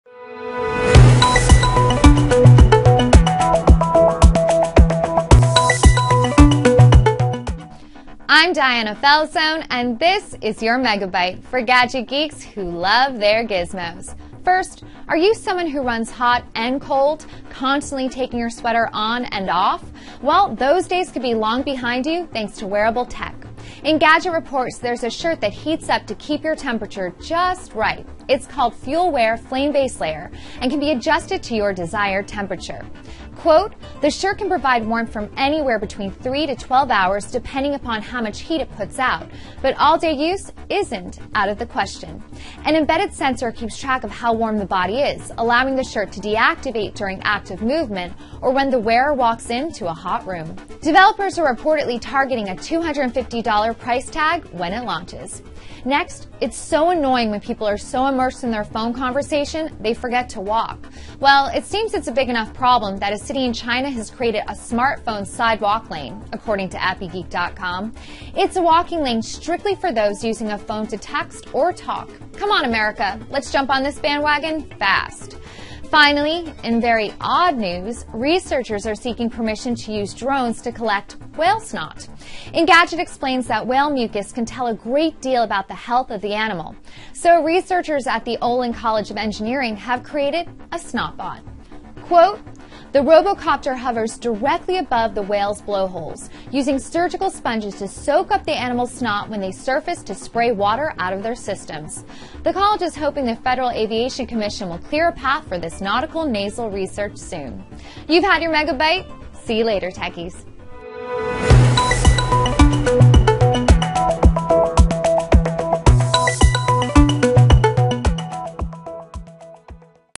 High tech shirt generates heat
Are you someone who runs hot and cold? Constantly taking your sweater on and off? Well, those days could be long behind you thanks to wearable tech.